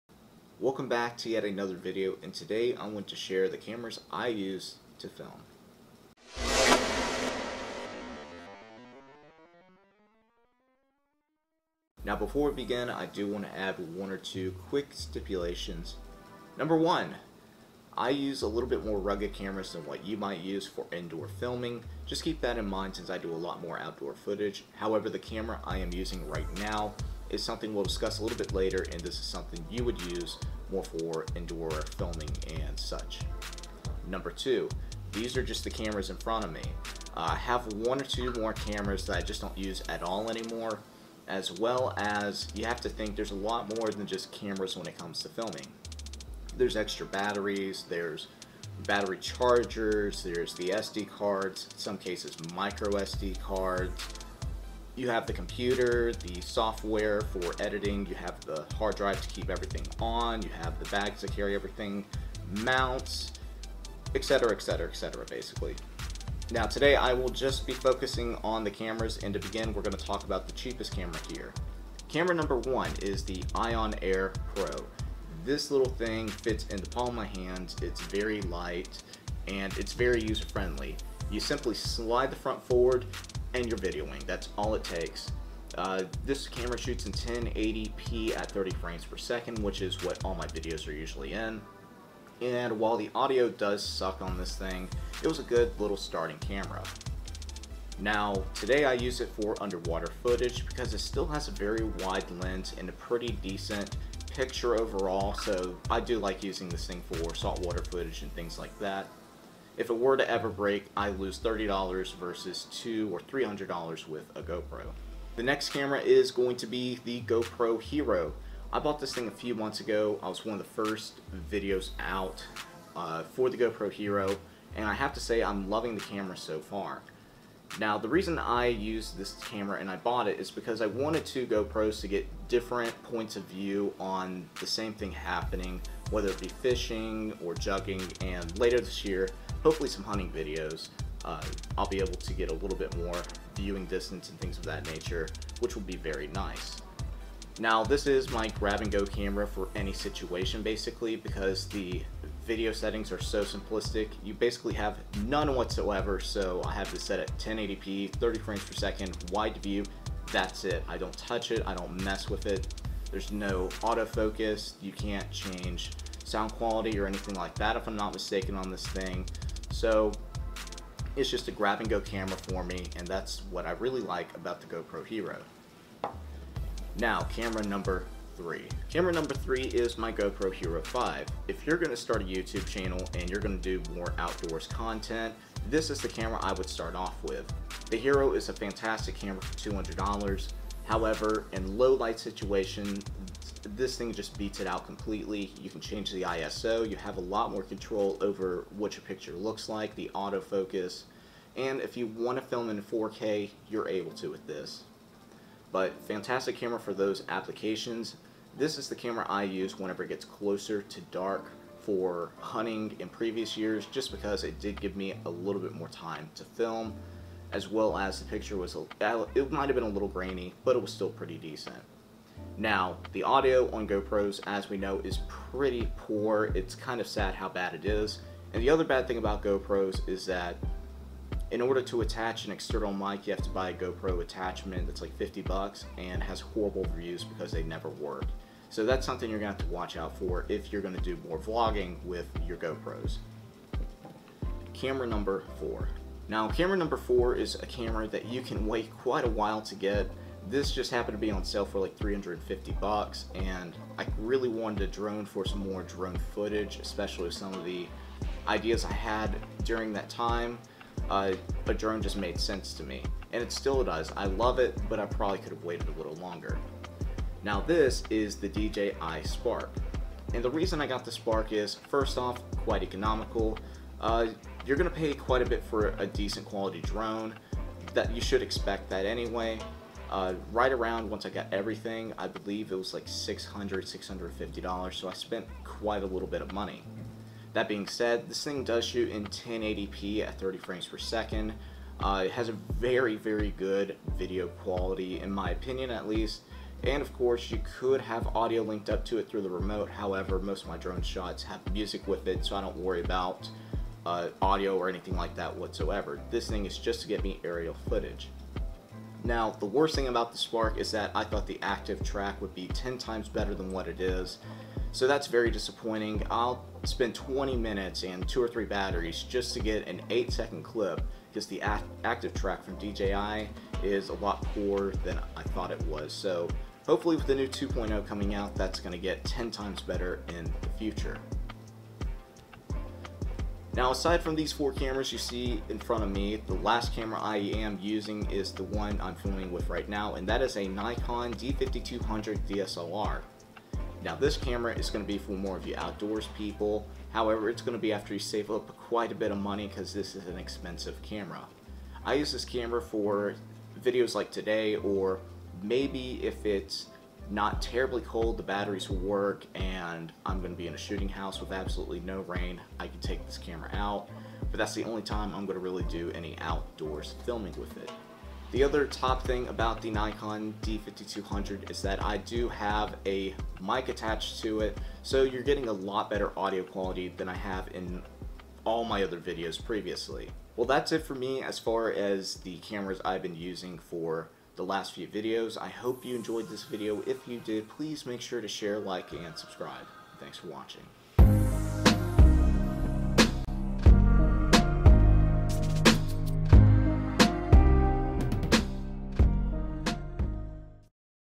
cameras for filming outdoor youtube videos
These are the cameras I use in order to film outdoor content for Youtube. The ion air pro, go pro hero 5, go pro hero, dji spark, and nikon d5200